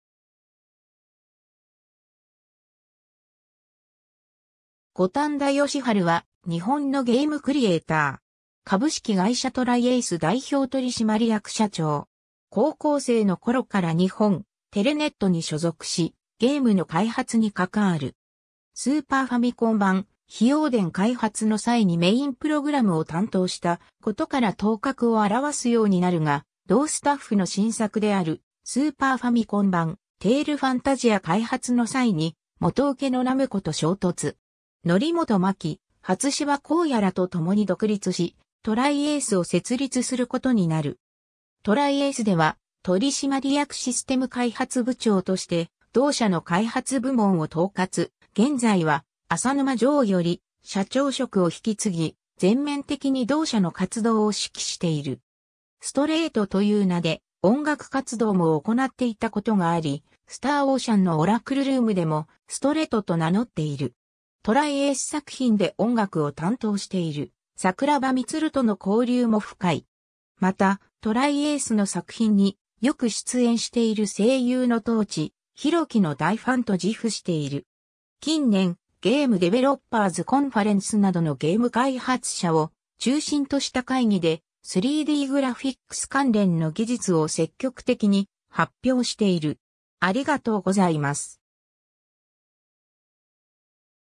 五反田義治
五反田 義治（ごたんだ よしはる、1976年 - ）は日本のゲームクリエイター。
株式会社トライエース代表取締役社長。
 高校生の頃から日本テレネットに所属し、ゲームの開発に関わる。
 スーパーファミコン版『緋王伝』開発の際にメインプログラムを担当した事から頭角を現すようになるが、同スタッフの新作であるスーパーファミコン版『テイルファンタジア』（のちに名称変更し、『テイルズ オブ ファンタジア』として発売）開発の際に、元請けのナムコと衝突。
則本真樹、初芝弘也らと共に独立しトライエースを設立する事になる。
 トライエースでは取締役システム開発部長として同社の開発部門を統括、現在は浅沼穣より社長職を引き継ぎ、全面的に同社の活動を指揮している。
 「STRAIGHT」という名で音楽活動も行っていたことがあり、スターオーシャンのオラクルルームでも「すとれと」と名乗っている。
トライエース作品で音楽を担当している桜庭統との交流も深い。
また、トライエースの作品によく出演している声優の東地宏樹の大ファンと自負している。
 近年、Game Developers Conferenceなどのゲーム開発者を中心とした会議で、3Dグラフィックス関連の技術を積極的に発表している。